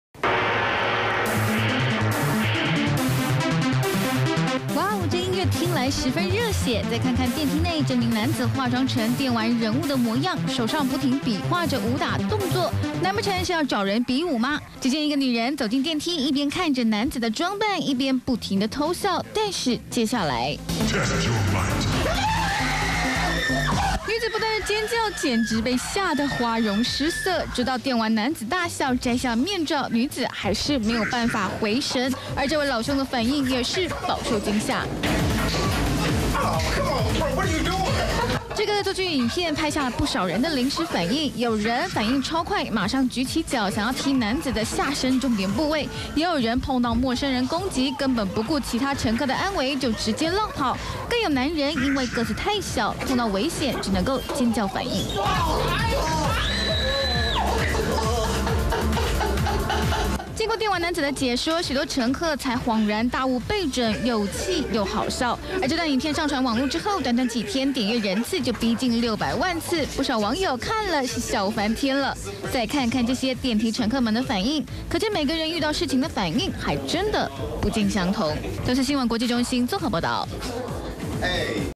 電梯遇真人快打 門一關就要戰鬥
電梯遇「真人快打」門一關就要戰鬥2014年09月17日當你搭乘電梯時，突然遇到一個知名電玩《真人快打》裡的角色要跟你拚輸贏時，你會有什麼反應？

國外網友日前在YouTube放上一段惡作劇影片，只見一名男子在電梯裡裝扮成《真人快打》裡的角色，只要戰鬥聲一下就立即與搭乘電梯的人進行格鬥，有女子因此遇到嚇得花容失色，驚聲尖叫的狂按電梯開門，也有女子反應超快，立馬想踹男子下體回擊，有男子遇見危險後丟下同伴自己跑走，還有男子嚇到發出宛如豬叫的聲音，令看到影片的網友狂笑，直說「好棒啊」。

電梯《真人快打》影片放上YouTube短短3天，就已吸引逾530萬人次點閱，不少網友笑翻，看完後笑到「流淚」，還有網友正經的回應，這代表每個人遇到事情的反應都不同。